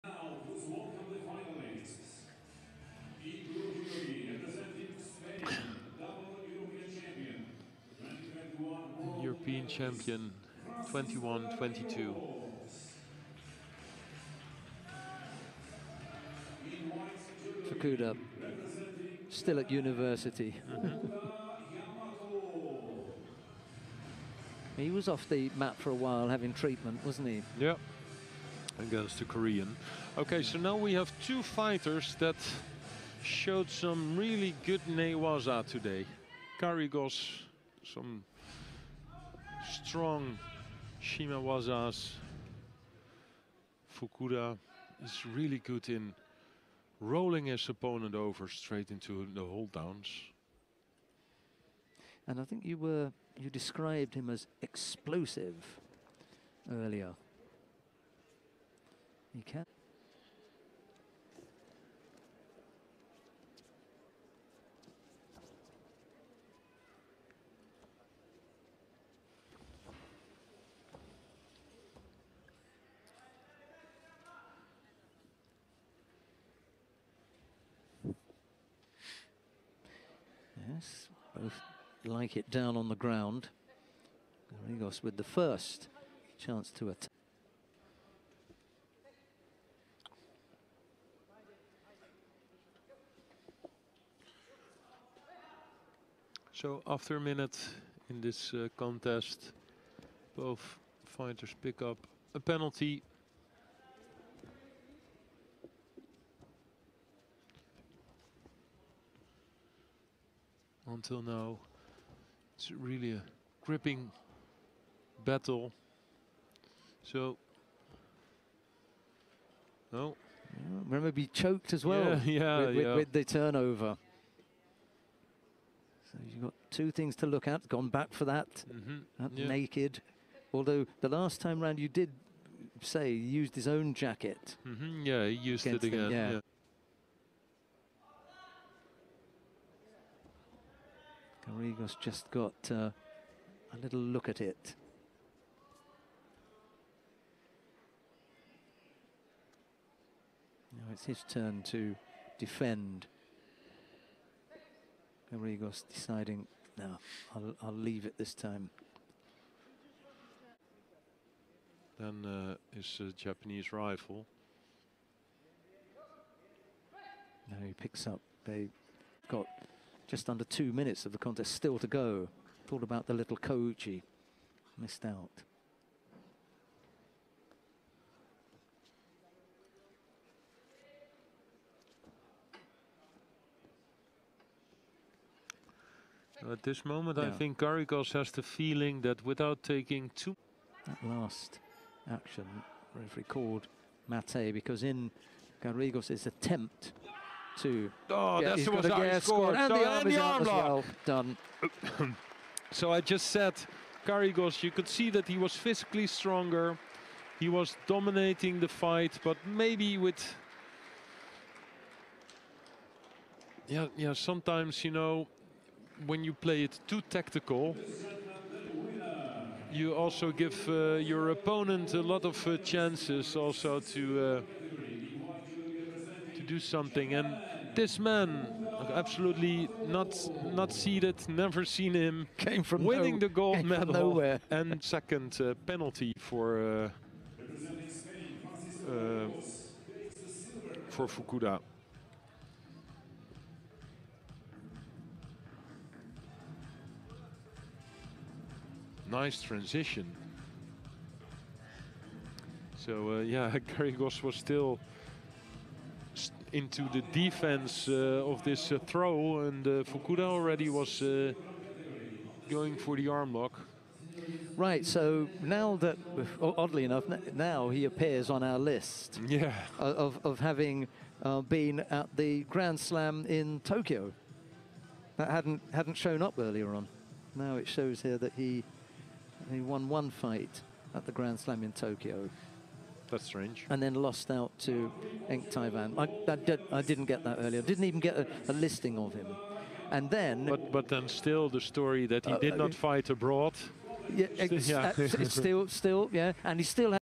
Yamato Fukuda vs Francisco Garrigós | Final -60 Grand Prix Portugal 2023
FloGrappling!
The best jiu-jitsu and grappling events.
Live or on-demand.

Yamato Fukuda
Country: Japan | Born: 12 February 2004 (18 years)
Judoka Yamato Fukuda of Japan finished fifth at the Cadet World Championships Almaty in the category U66 in 2019. He claimed a gold medal at the Junior European Cup in Malaga in 2022.

Francisco Garrigós
Country: Spain | Born: 9 December 1994 (28 years)
Francisco Garrigós became European Champion in 2021 and 2022. He took bronze at the European Championships in 2020. Fran bagged a world bronze medallist in 2021 in Budapest. Garrigos won European bronze in 2017 in Warsaw and silver in Minsk in 2019. Fran won the Grand Slam of Abu Dhabi in 2016 and Grand Prix in Cancun in 2017. He became World Junior Champion in 2014 in Miami and was also European Junior Champion Judo that year. He won the European Open in Lisbon in 2017. Fran claimed bronze at the Grand Slam in Antalya in 2022. He took silver at the IJF World Masters in 2019 in Qingdao.